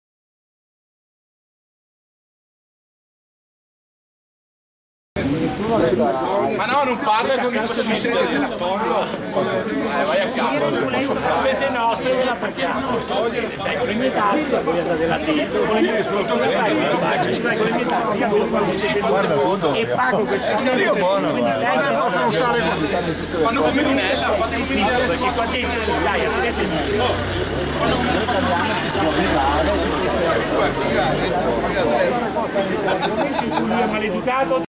Neofascisti contestano Kersevan a Montebelluna, 9 febbraio 2013.
Il 9 febbraio 2013 la storica Alessandra Kersevan era stata invitata a Montebelluna per una conferenza sui crimini di guerra italiani in Jugoslavia. Dalle pagine facebook di Forza Nuova Treviso era stato lanciata la parola d'ordine: "la Kersevan non deve parlare". Quindi una squadraccia di neofascisti si è presentata davanti alla sede dell'Anpi di Montebelluna.

al link sotto indicato l'articolo con la cronaca di una giornata di ordinario neofascismo: